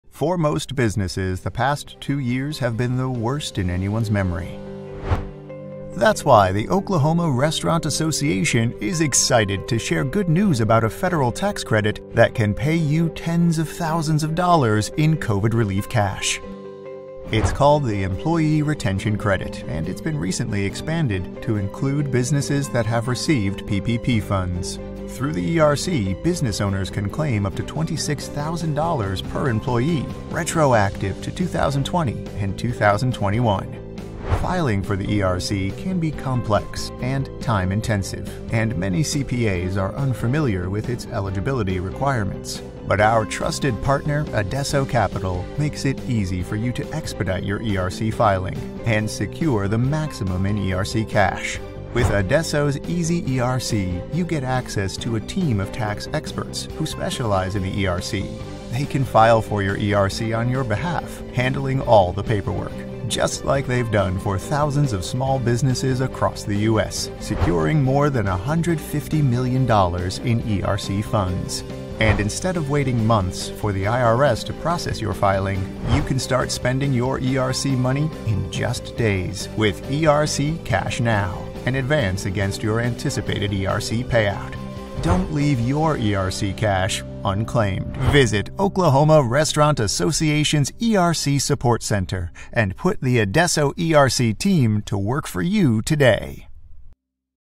Adesso Capital - OKRA ERC Introduction
Restaurant Relief™ is an Oklahoma Restaurant Association program that brings our restaurants and lodging operators the financial assistance they need right now.

Some of that assistance will come from a federal initiative called the Employee Retention Credit (ERC). You may qualify for up to $26,000 per W-2 employee.

This is like a grant for your business, and it doesn't need to be repaid. And you may be eligible even if you received a PPP.

to get started.